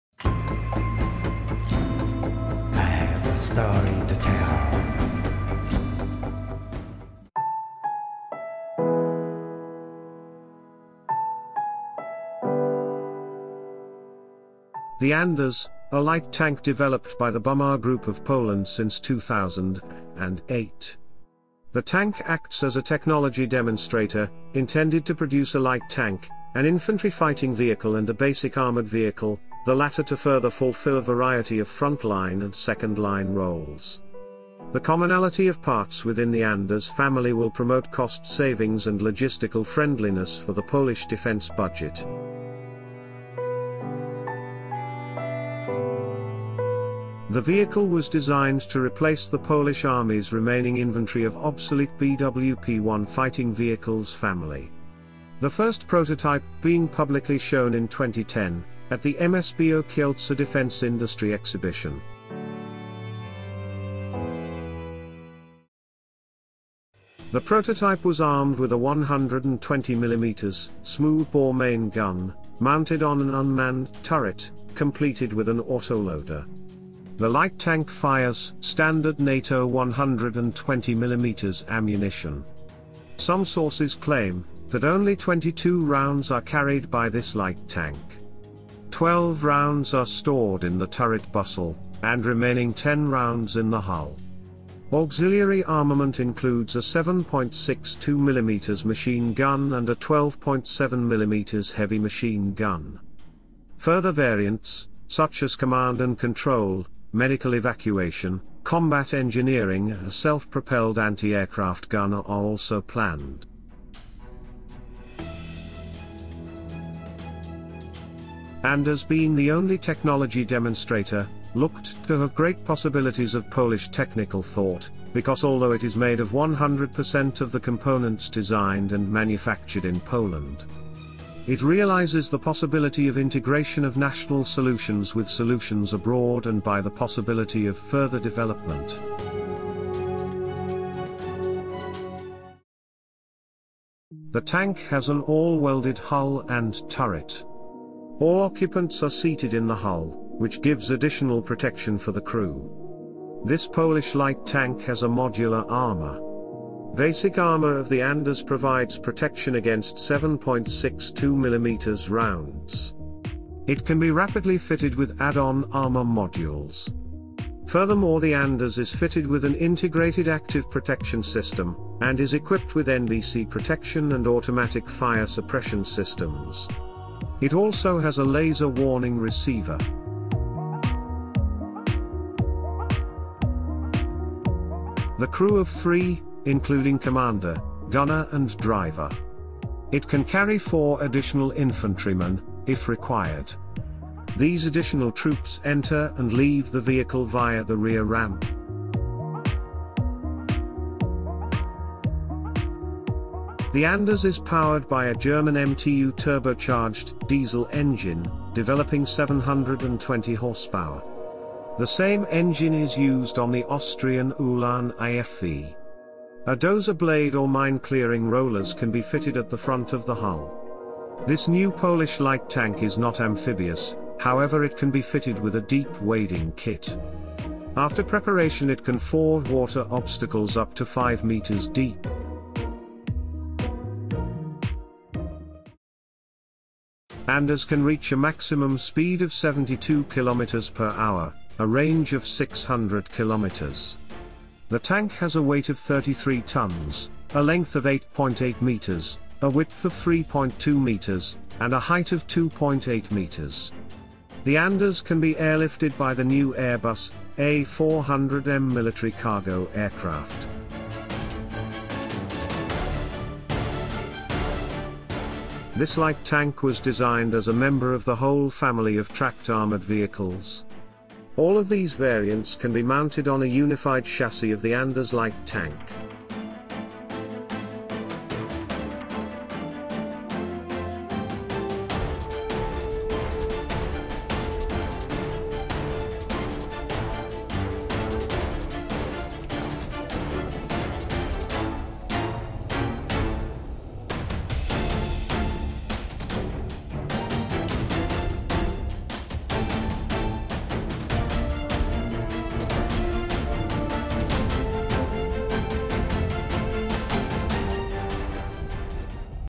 WPB Anders: A Light Tank Demonstrator From Poland
The WPB Anders, a light tank developed by the Bumar group of Poland since 2008. The WPB Anders tank acts as a technology demonstrator, intended to produce a light tank, an infantry fighting vehicle and a basic armored vehicle, the latter to further fulfill a variety of front-line and second-line roles. The commonality of parts within the Anders family will promote cost savings and logistical friendliness for the Polish defense budget.